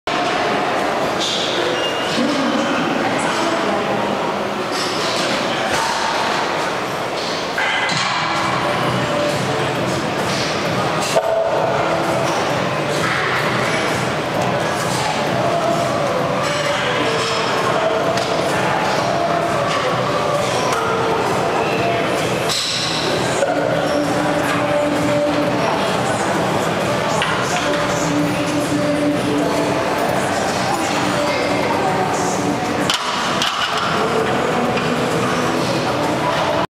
155x5 Squat
03/15/2011
Form check - sticking point. :(
Gear - leg sleeves.